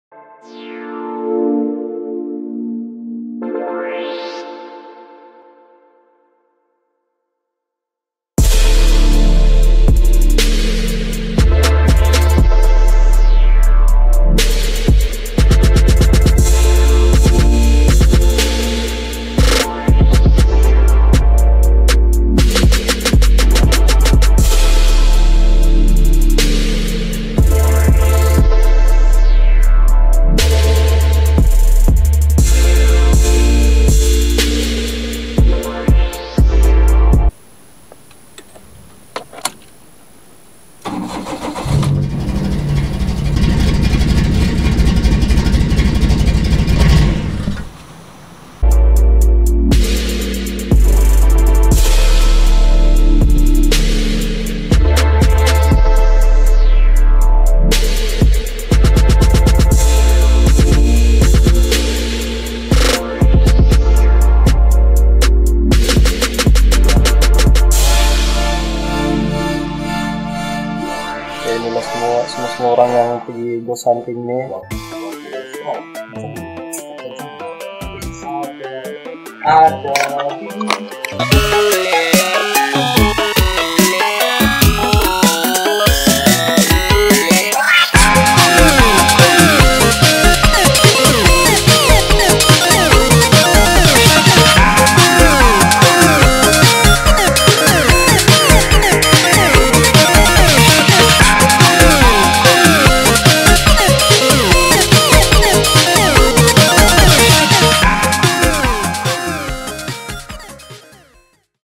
ELITE RS 72.4%
Custom Elite RS, coming soon front diffuser splitter rear bumper cross-breed with myvi icon 1.5 and rocket bunny fender flair wide body + sport rims. Soon gonna repair and repaint send to workshop do some touch up and just bought new suspension. Elite RS still in progress 72.4%